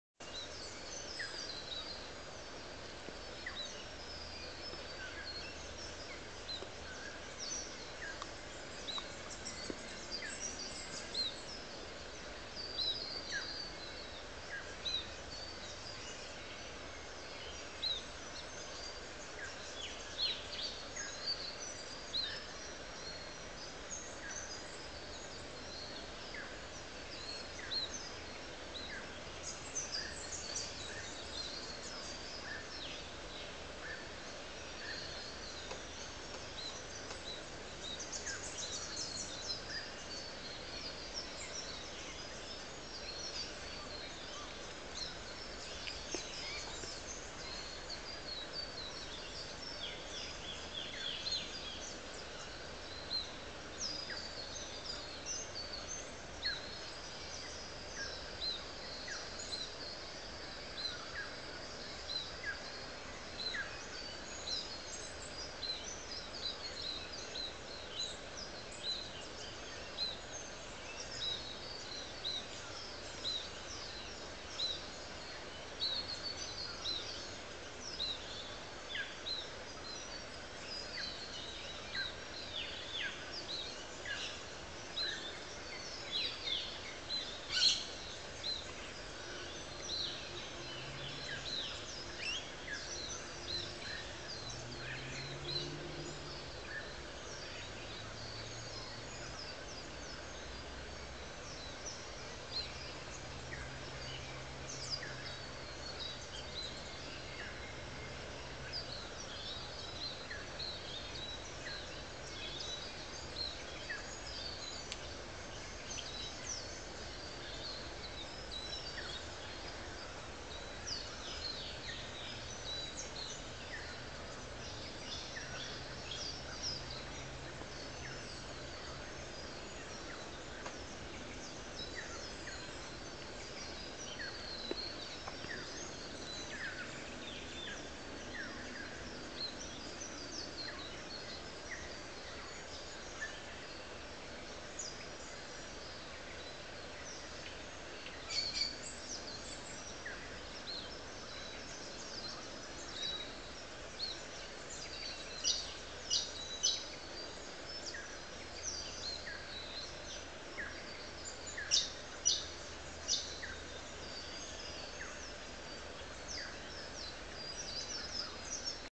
Double-barred Finches Building a Nest
A pair of Double-barred finches (Taeniopygia bichenovii) building a nest in the orchard at Tondoon Botanic Gardens in Gladstone, Queensland.